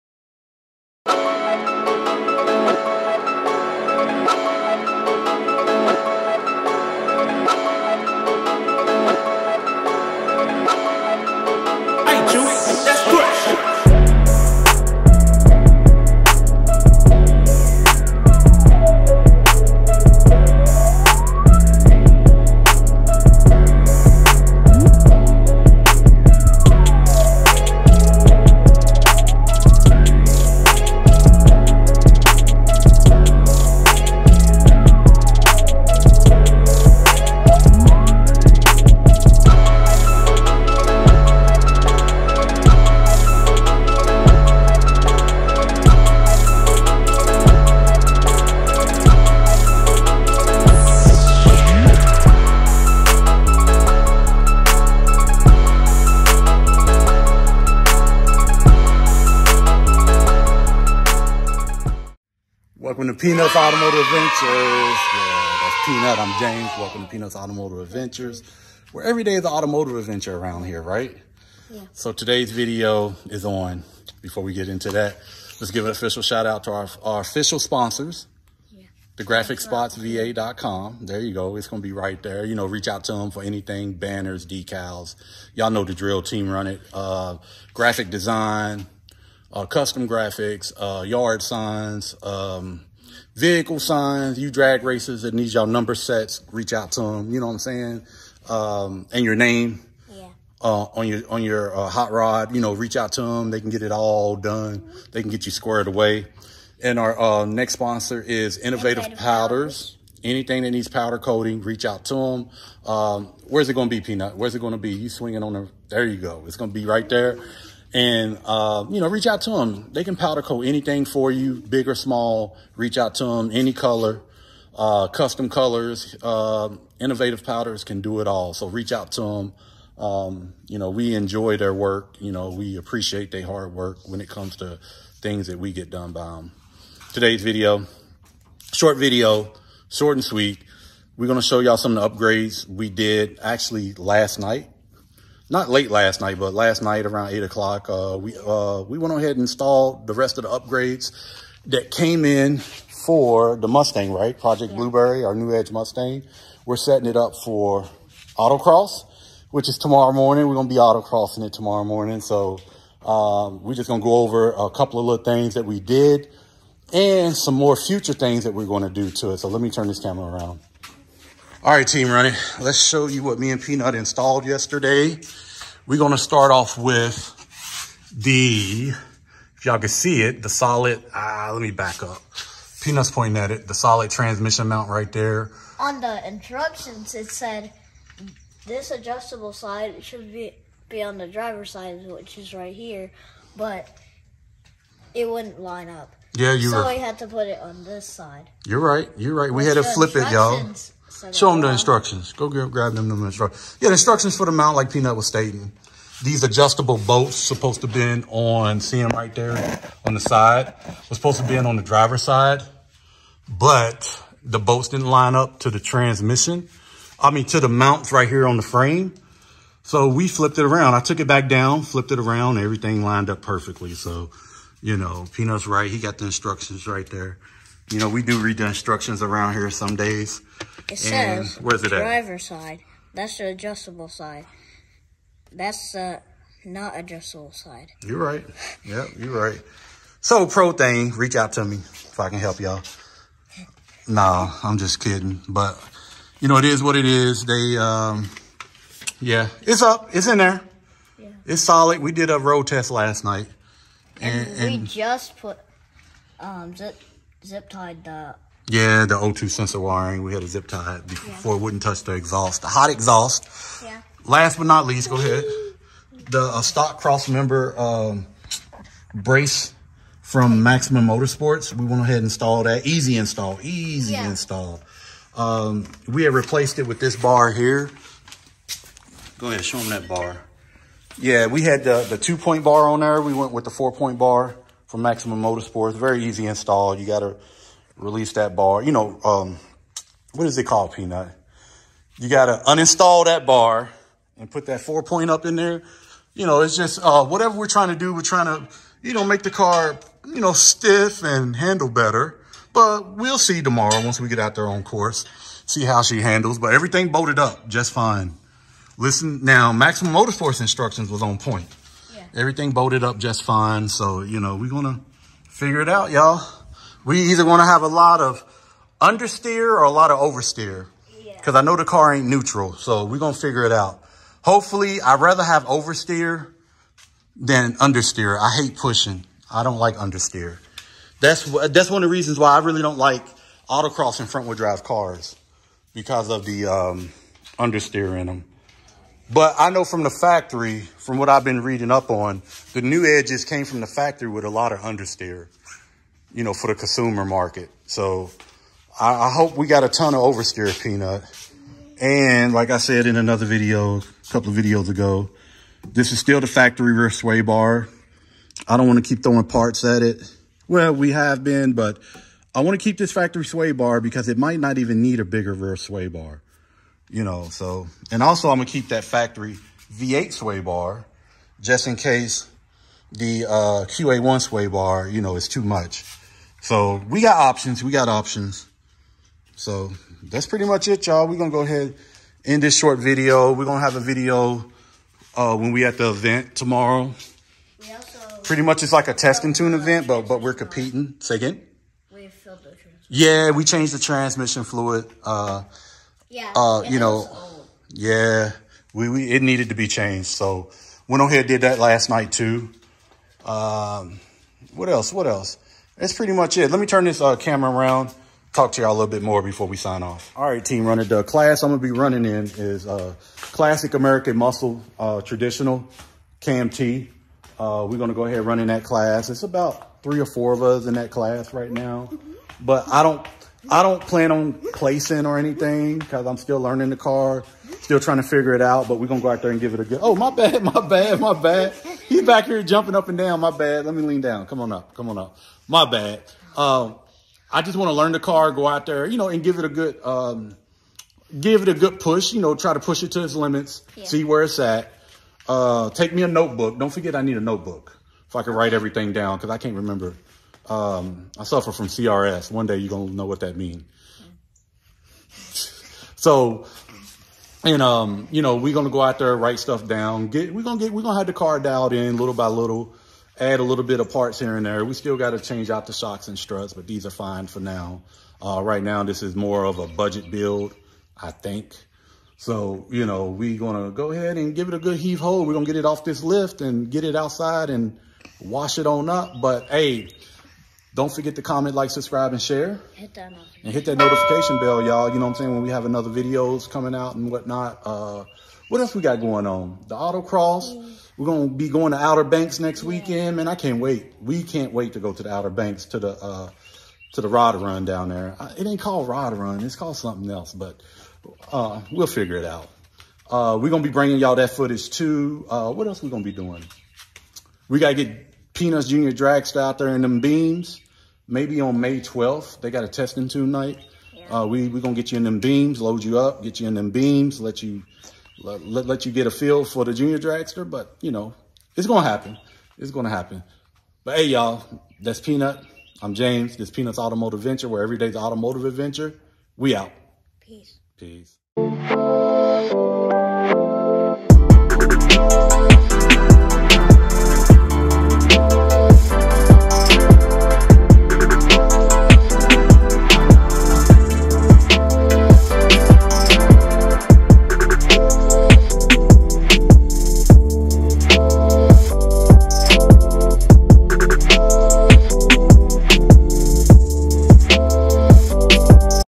Preparing for our 1st Autocross for 2024
We are prepping our 2001 Ford Mustang GT for the SCCA Old Dominion Autocross at South Hampton Speedway tomorrow in Capron, Virginia.